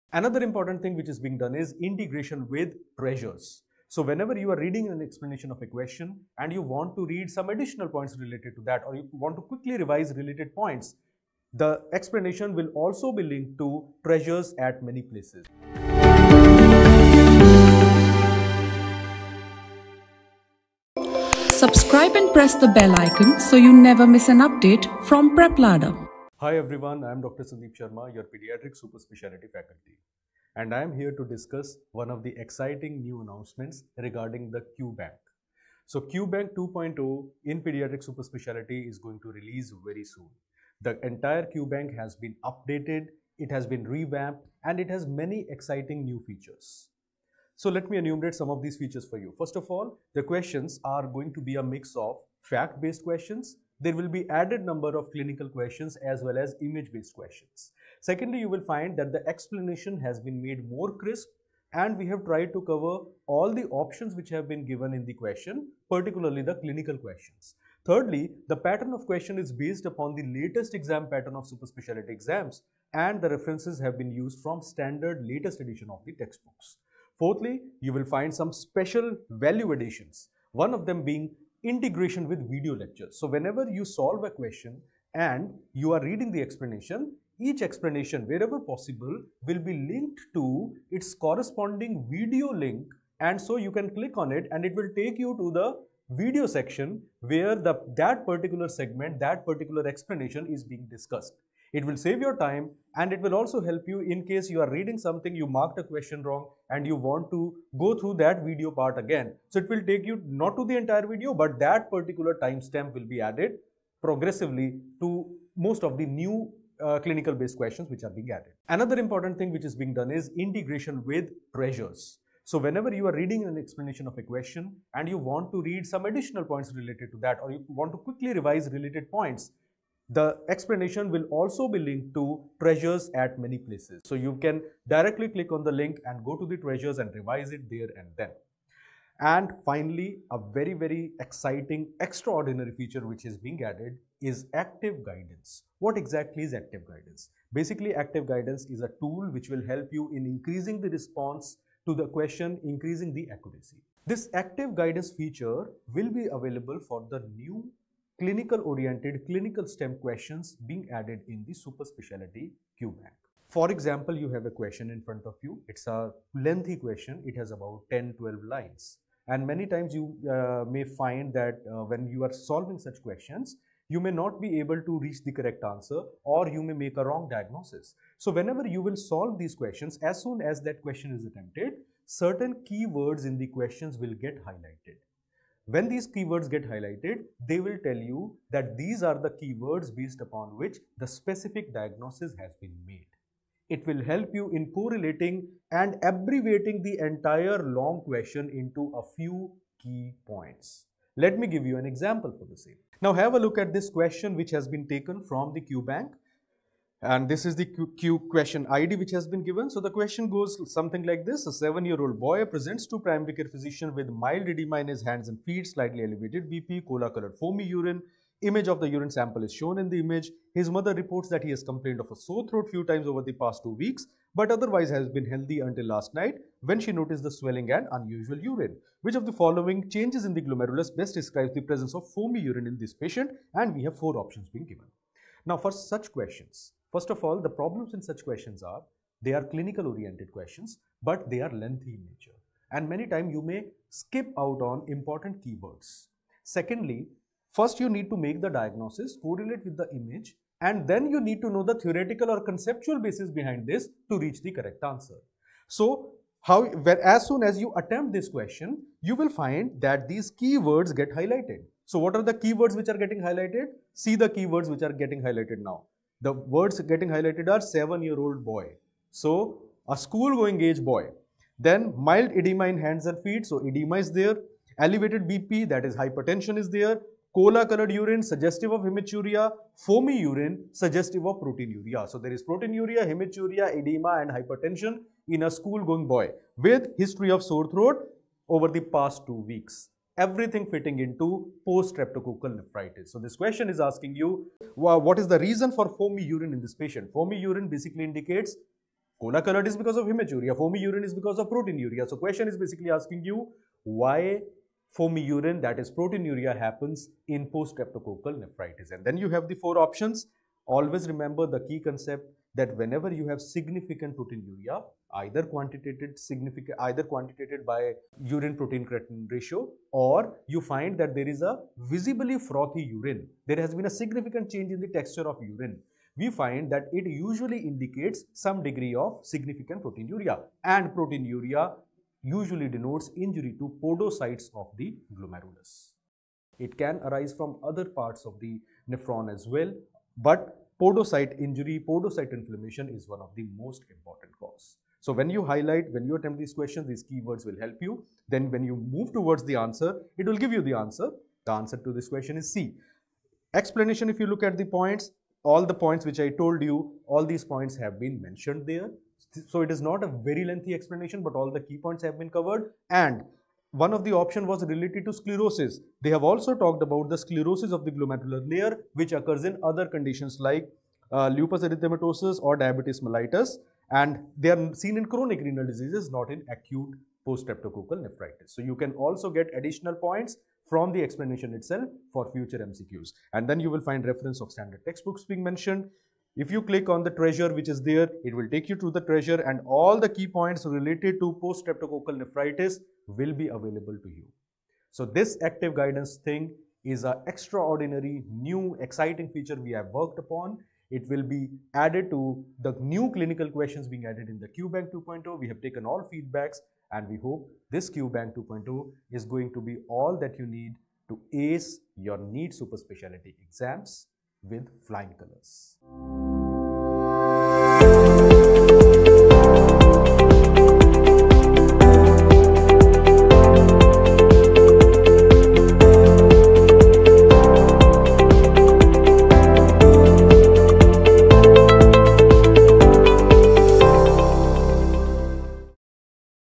High Yield Question Discussion | QBank 2.0 | Dr. Sandeep Sharma | NEET SS 2.0
Watch High Yield Question Discussion by PrepLadder's NEET SS Faculty for Pediatrics - Dr. Sandeep Sharma.

In this video, he will shed light on:

- Why QBank 2.0 is best for NEET SS preparation
- Amazing Features of QBank 2.0
- Integration of QBank 2.0 with Treasures
- Clinical Questions in QBank 2.0
- QBank 2.0 Based on Latest NEET SS Pattern
- How to attempt questions in NEET SS?
- What is the reason for foamy urine?
- Why does Proteinuria happen?

Enjoy 7-day free access to Cardiovascular (Medicine), Urology (Surgery) & Pediatric Gastroenterology and Hepatology (Pediatrics)